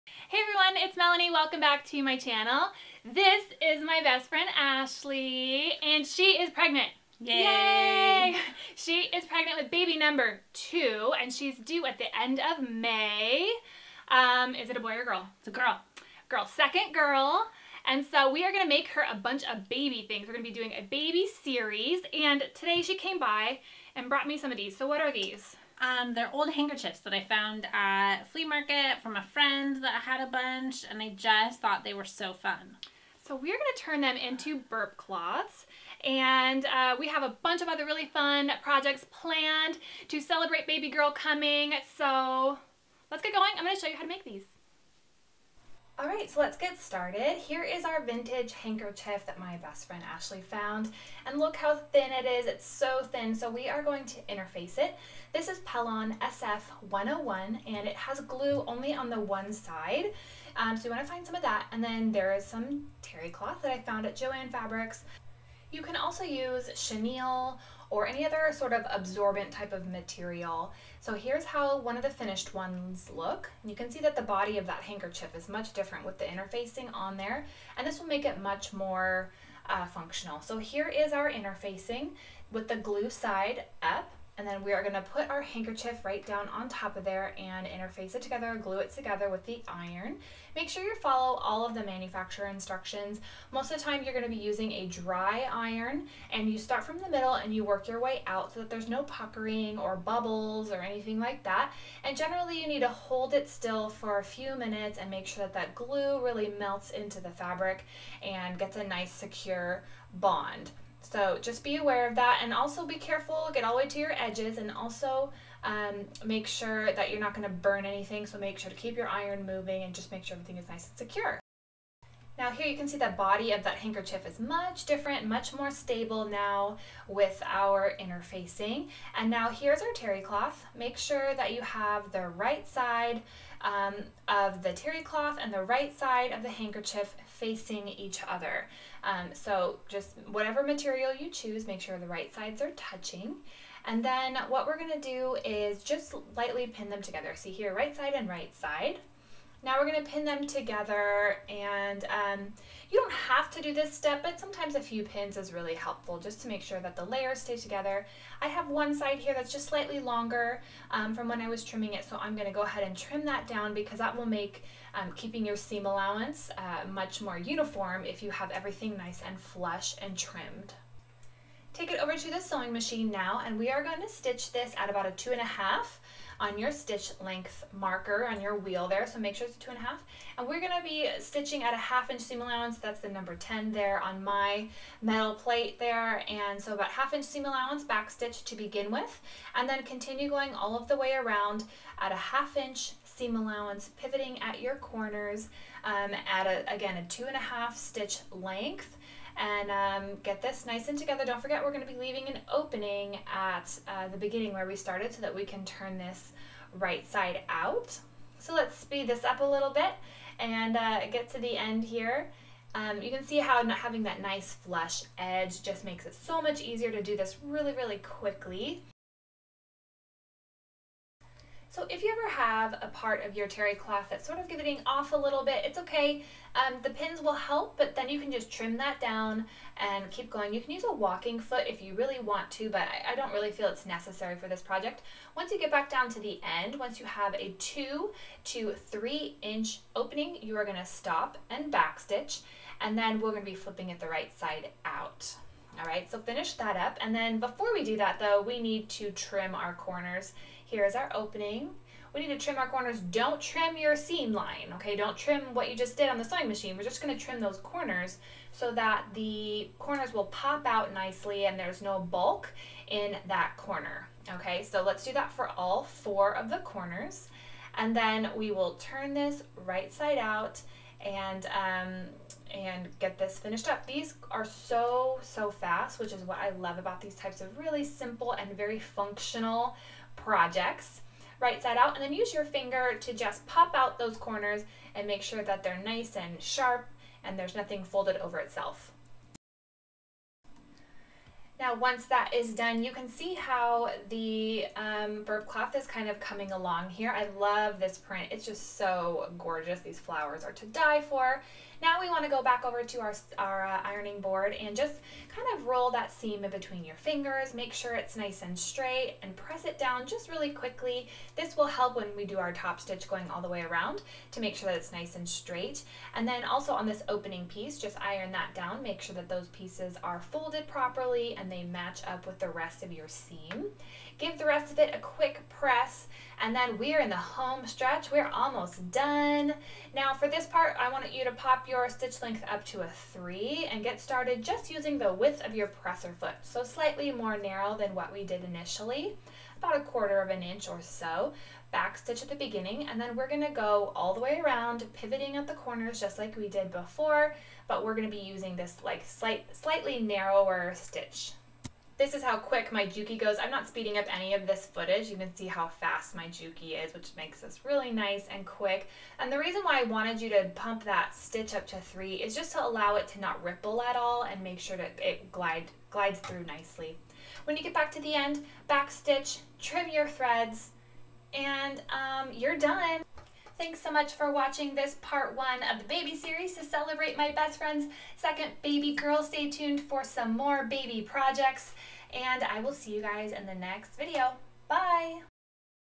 How to Make Burp Cloths from Vintage Hankies: Mel's Baby Series
Today we are going to make some burp cloths from vintage handkerchiefs to kick off my new baby series!  This sewing tutorial is very easy, beginner friendly and helps to use some really pretty prints in a functional way!

Materials:
Pretty Handkerchiefs (If you don't have any or don't want to use those, use some cotton prints.  If its not too thin you can skip the interfacing step)
Pellon SF101 for interfacing
Terry Cloth (chenille, cloth diapers work as well)
Coordinating thread
Iron and Ironing board
Pins

Some Favorite Sewing Supplies

Fiskars Scissors

Rotary Cutter and Mat Set

My Juki Sewing Machine